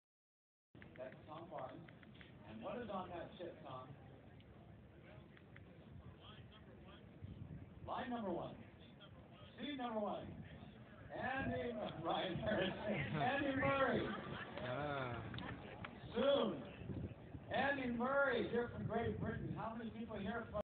andy murray name drawn 1st 3/7/17 MAIN DRAW CEREMONY, INDIAN WELLS BNP PARIBAS 2017 excerpt
Greetings ! I INVITE U 2ACCEPT JESUS CHRIST as your Lord & Savior & be Saved TODAY !